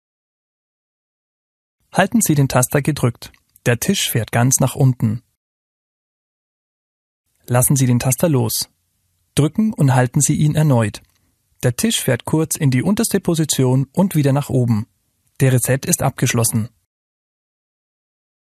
Reset Sitz-Steh-Tisch VXDLR
In diesem Video beschreiben wir den Reset des Sitz-Steh-Tisches VXDLR Schritt für Schritt.